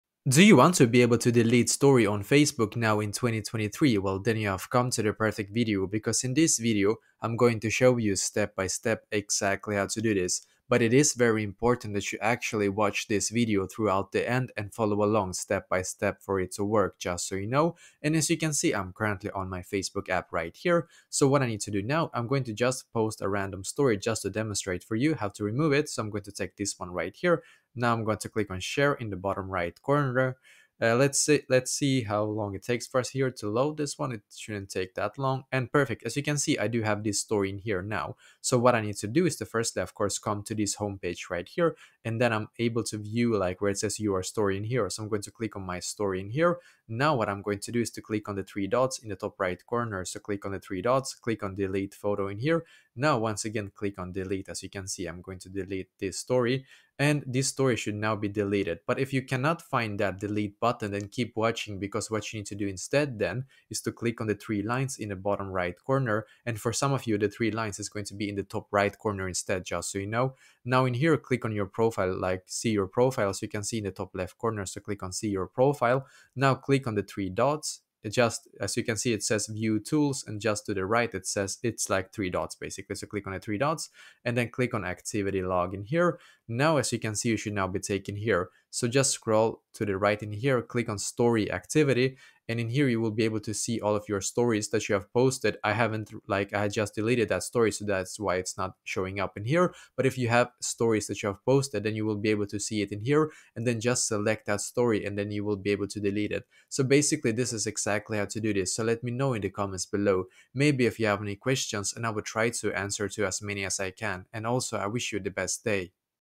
How To Delete Story On Facebook 2025
If you want to delete story on facebook then this video will help you with this!